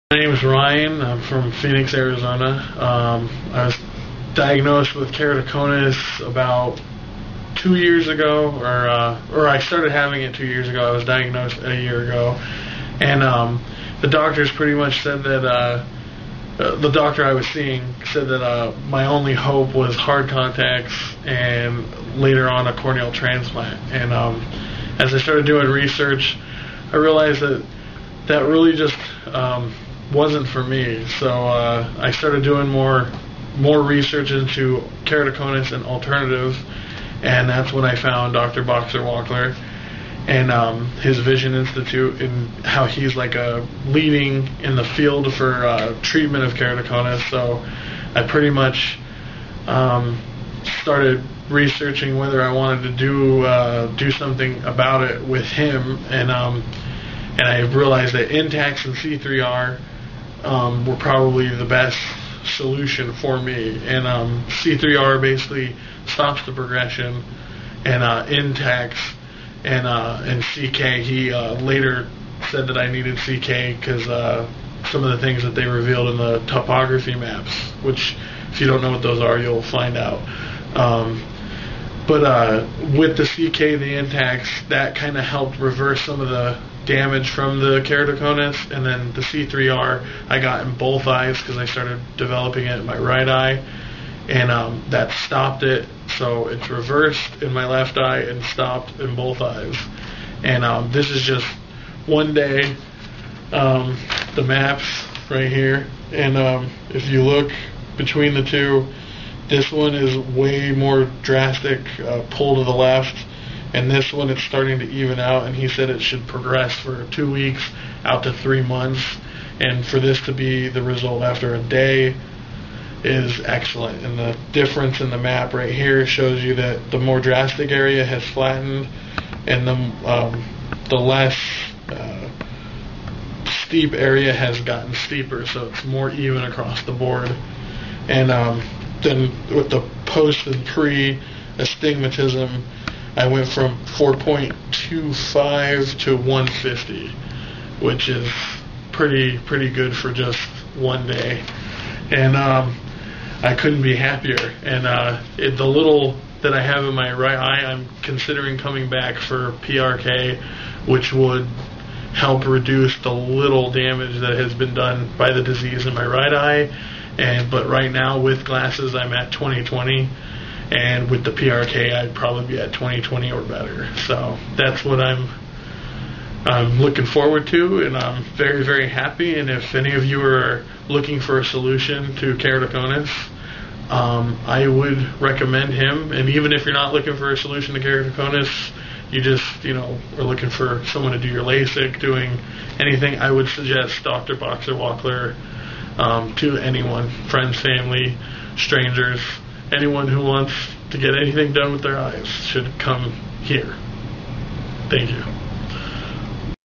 Keratoconus Story : Intacs, C3-R & CK Experience with Dr. Brian Boxer Wachler
Ryan talks about his Holcomb C3-R, Intacs, and CK experience at Boxer Wachler Vision Institute.